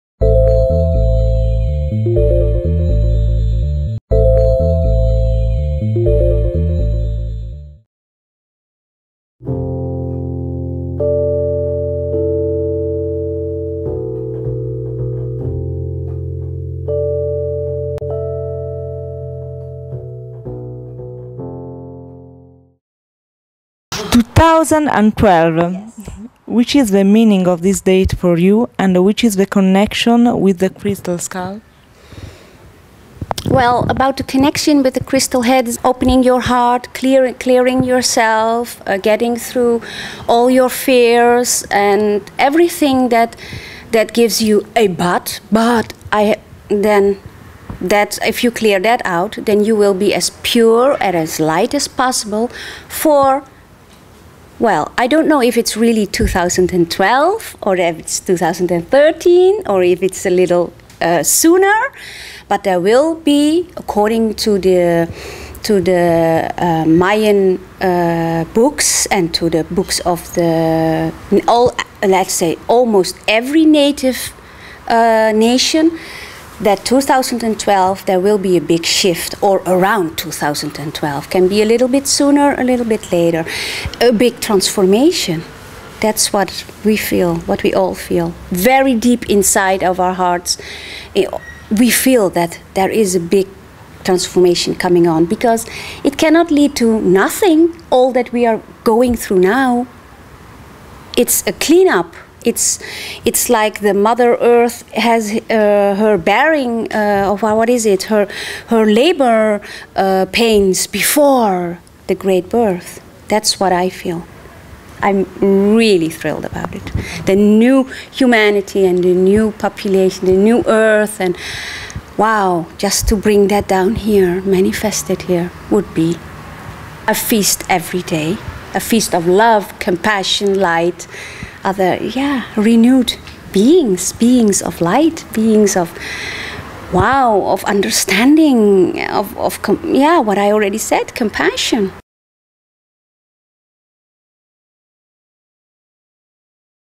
2012: cosa ci sta aspettando?
Risponde Blue Arrow Rainbow - Video realizzato al Convegno "Il Mistero del Teschio di Cristallo" organizzato da Acacia Edizioni, Milano 5 luglio 2008.